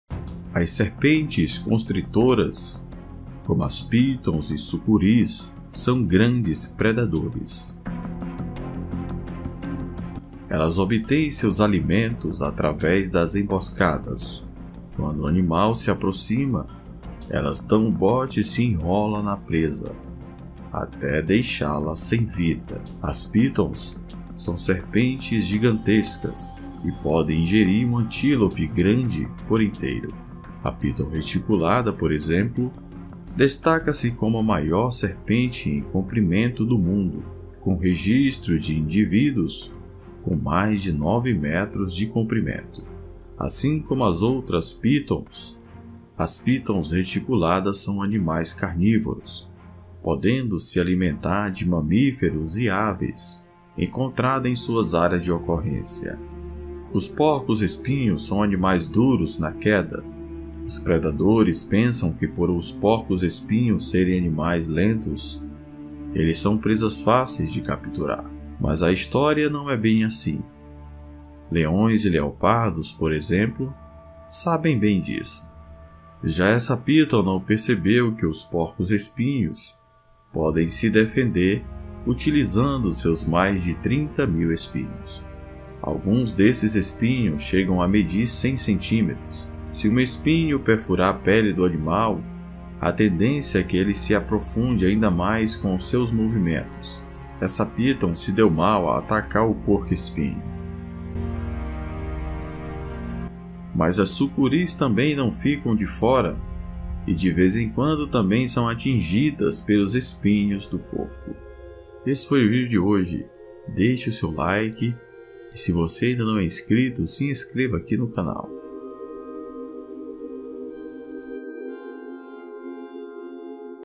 COBRA ATACA PORCO ESPINHO E SE DÁ MAL
Veja neste vídeo o que aconteceu com uma cobra que atacou um porco espinho. a cobra atacou o porco espinho e se deu mal,ela tambem levou a pior Conheça  nesse vídeo também várias curiosidades sobre o porco espinho. Cobra ataca porco espinho e leva a pior.

Aqui no canal quefatos você vai encontrar vídeos de todos os tipos de animais , descobrir curiosidades, notícias e muito mais.